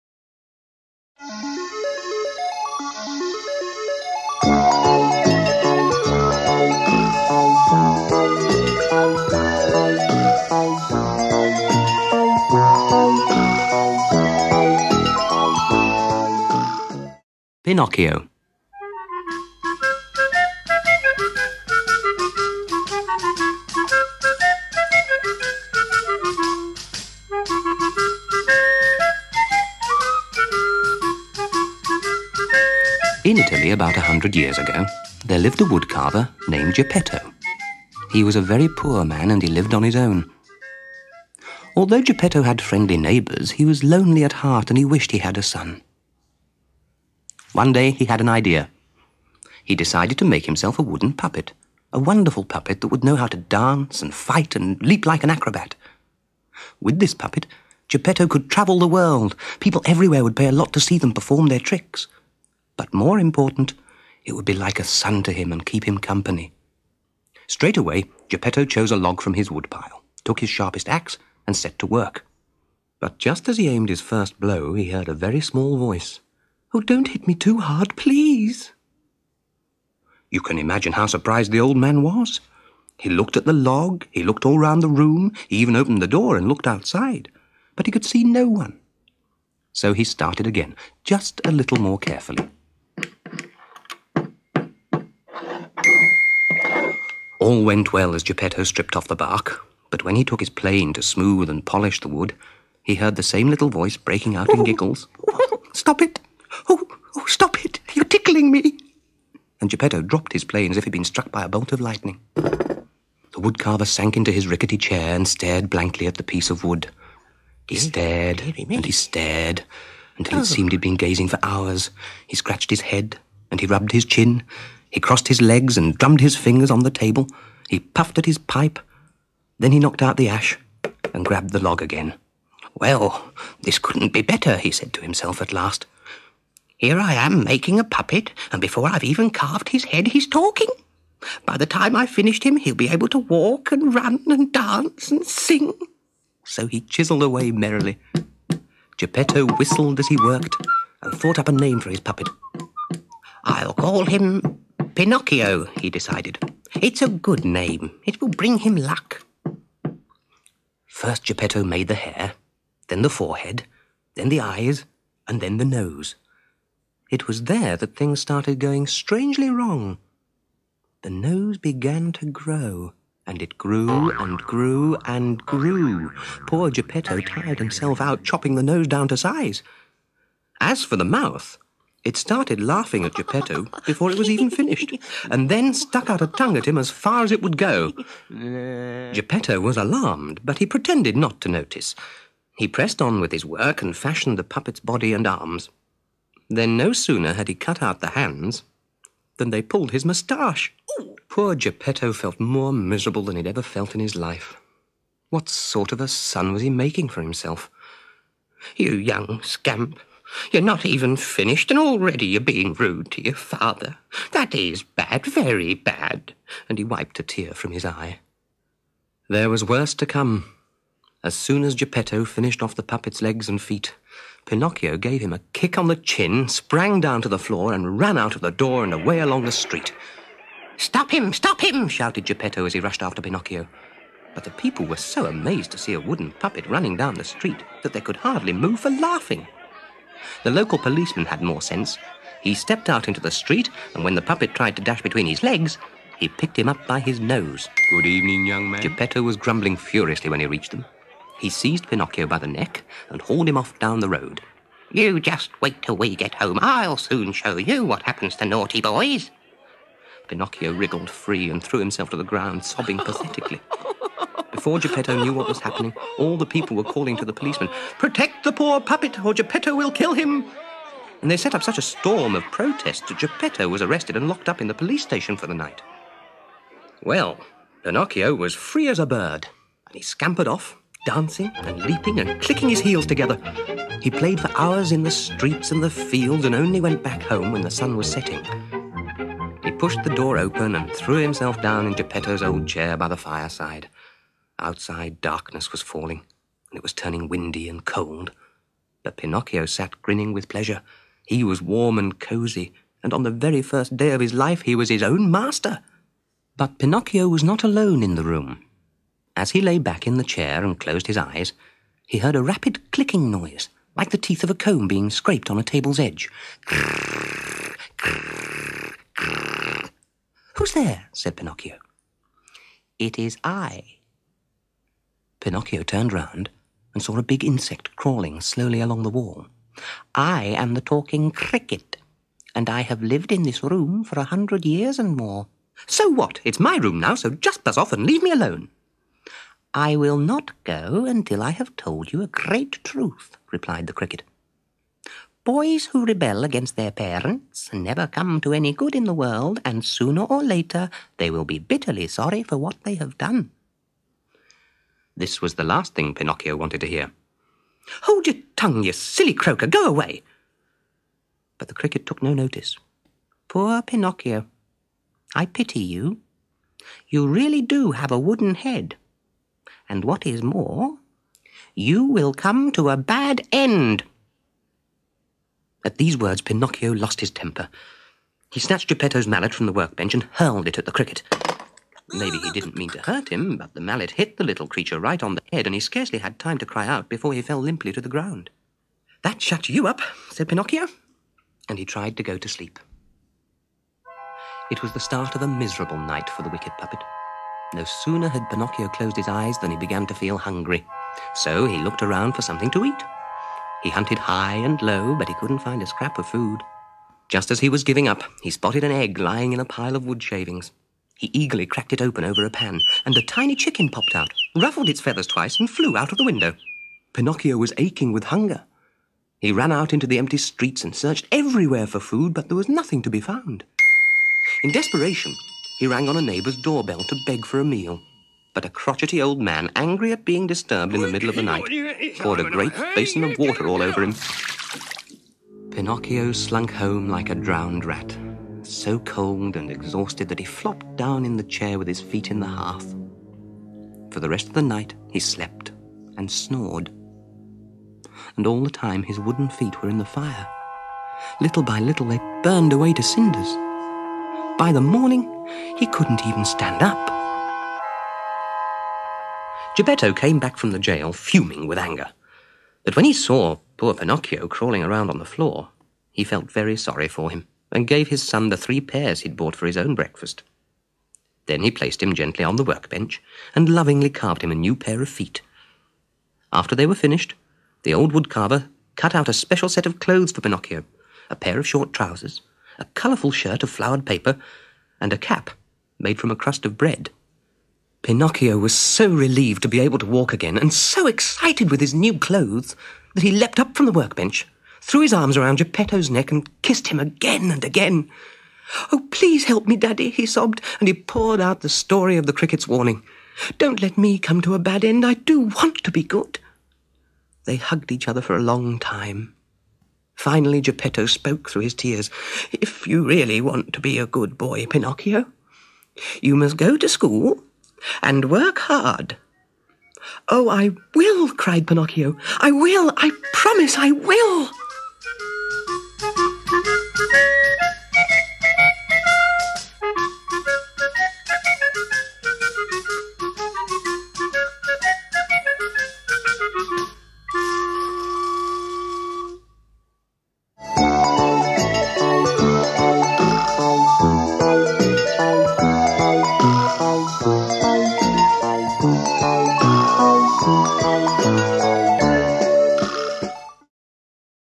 Pinocchio - Part 1
0:16 - The first chapter of an ambitious new telling of the Italian classic. In preparing it, John Ford has returned to Carlo Collodi's original book, since much of the story's action and flavour has been lost by animators and adaptors. Story Teller's Pinocchio has been drawn in traditional style by Francis Phillipps -- and even the puppet's name is pronounced on the tape the Italian way.
8:45 - Based on the story by Carlo Collodi, Adapted by John Ford, Illustrated by Francis Phillipps, Read by Ian Lavender